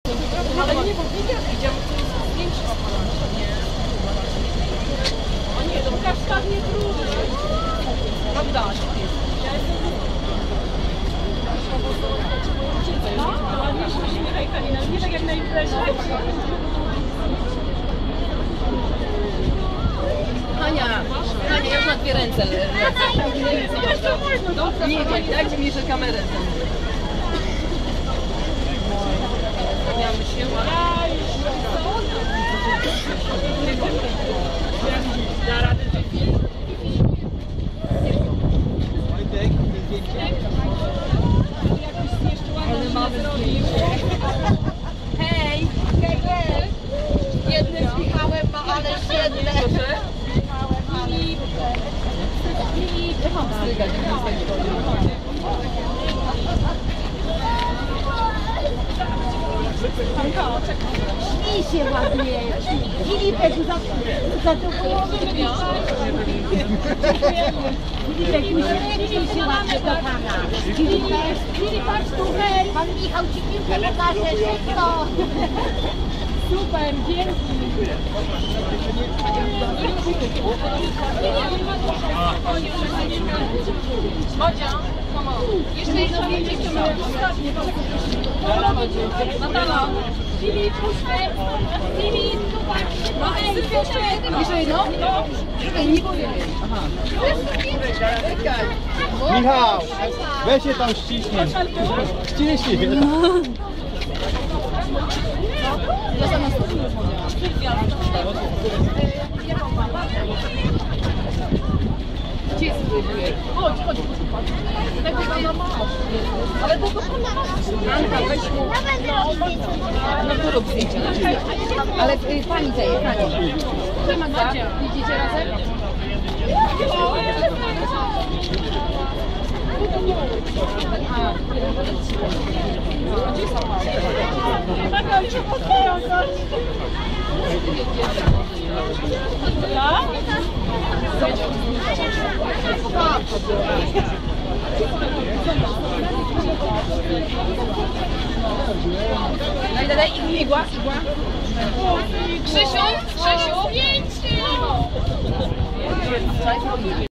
Polscy siatkarze po treningu w Sulechowie 14.07.2012 [6]
Chłopaki po treningu przed Memoriałem Wagnera (w Zielonej Górze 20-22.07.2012).
Michał Winiarski, Bartosz Kurek i fani.